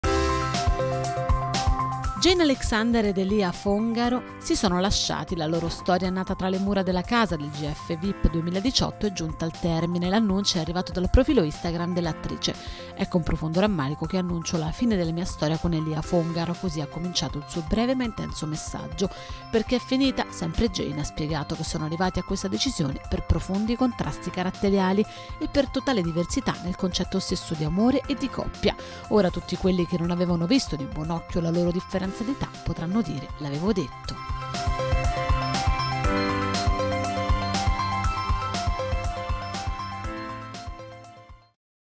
Jane Alexander ed Elia Fongaro - Ecco perché si sono lasciati!
La storia d'amore nata all'interno della casa del Grande Fratello Vip è finita e Jane Alexander ne spiega i motivi con un post su Instagram